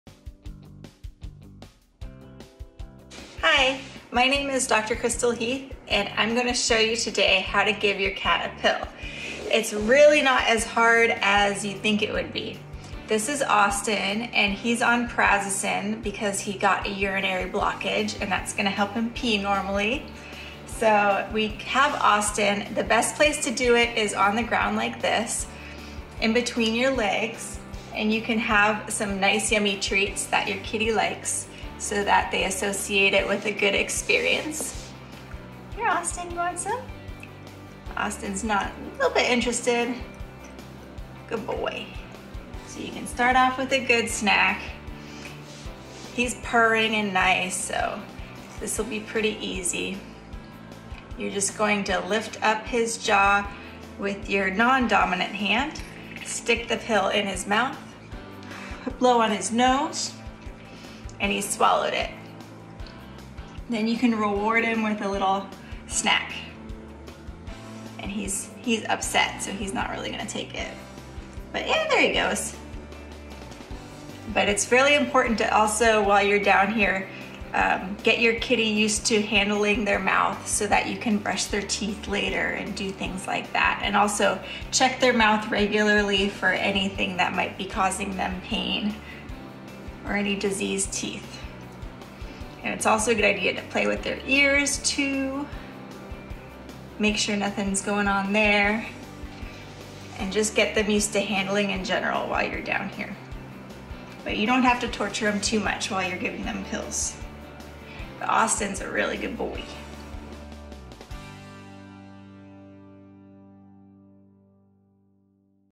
Training Tips: How to Give Your Cat a Pill
Dr. Crystal Heath demonstrates how easy it can be to give your cat a pill and why getting them used to being handled around the mouth and ears is a good thing! More Training Tips at berkeleyhumane.org